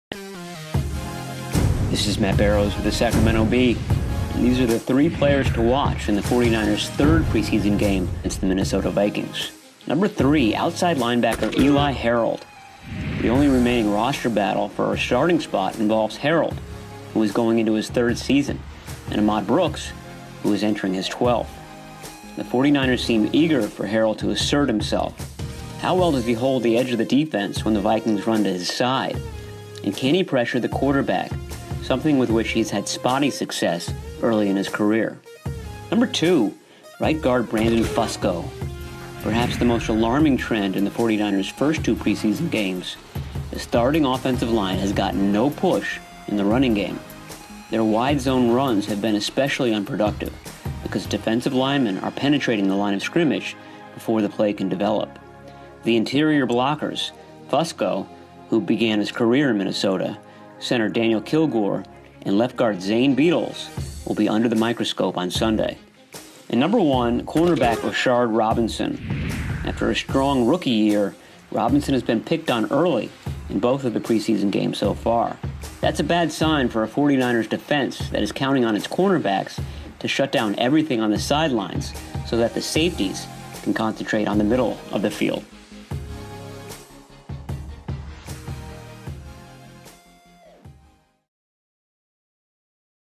Outside linebacker, guard and cornerback come into play in 49ers preseason
SANTA CLARA

The 49ers released the longest tenured player on their defense, outside linebacker Ahmad Brooks, the team announced Friday.

Brooks, 33, has been battling with third-year player Eli Harold for the starting strong-side linebacker spot and Harold started the most recent game against the Broncos.

Brooks was informed of his pending release Friday morning. Brooks either has led or tied for the 49ers’ team lead in sacks every season since 2013.

“We would like to thank Ahmad for his nine years of service and contributions to the 49ers,” said 49ers general manager John Lynch in a statement. “In that time, he played a significant role on some formidable defensive units that helped this team achieve great success. We feel good about the depth and competition along our defensive front and believe that by making this decision now it will give Ahmad a head start on exploring his options with other teams.”

San Francisco has been shopping him for weeks and has received some nibbles, although his age and hefty salary are deterrents.

Harold not only is 10 years younger than Brooks, no two players competing for the same spot have nearly as wide a discrepancy in their salaries. Brooks, who is entering his 12th season, is San Francisco’s fourth highest paid player, and his $6.1 million salary-cap figure dwarfs that of Harold – $823,000.

“Eli’s very athletic. I mean, they’re both built the same,” defensive coordinator Robert Saleh said Thursday when asked about Harold and Brooks. “If you’re looking at Eli and Ahmad, they’re very athletic, they’re both very strong, they both love the game. So it’s a good problem. You’ve got two very good football players.”

Harold also has two more years on his contract; Brooks is entering his final season. The 49ers also like newcomer Dekoda Watson, a special teams ace who plays strong-side linebacker on defense. Rookie Pita Taumoepenu also has played that spot as well as “Leo” pass rusher.

Brooks has been a starter on the edge of the team’s defense since 2011, and he’s the second longest tenured 49er after left tackle Joe Staley. He has 53 1/2 career sacks; Harold has three.

Staley, linebacker NaVorro Bowman, center Daniel Kilgore and tight end Garrett Celek are the only current 49ers who played in Super Bowl XLVIII.

As the 49ers key up for a preseason game against the Minnesota Vikings, The Sacramento Bee's Matt Barrows gives us three players to watch in this week's game.